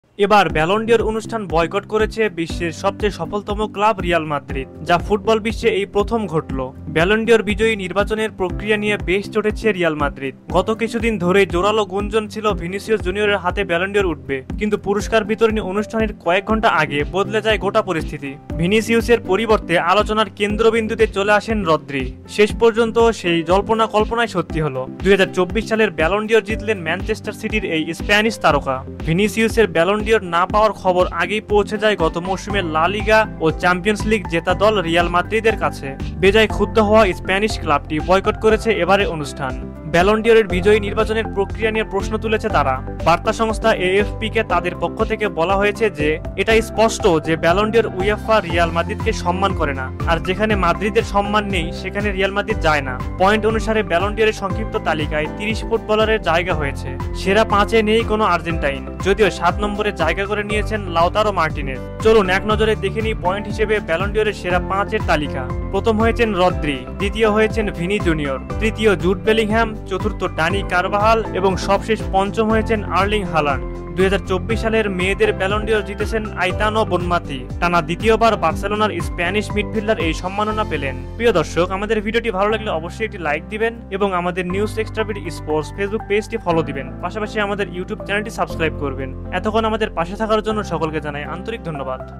ভিনিকে পিছনে ফেলে বিতর্কিতভাবে ব্যালন ডি’অর জিতলো স্প্যানিশ তারকা রদ্রি । | Ballon d'Or | Rodri
ভিনিকে পিছনে ফেলে বিতর্কিতভাবে ব্যালন ডি’অর জিতলো স্প্যানিশ তারকা রদ্রি । | Ballon d'Or | Rodri | Vinicius Junior |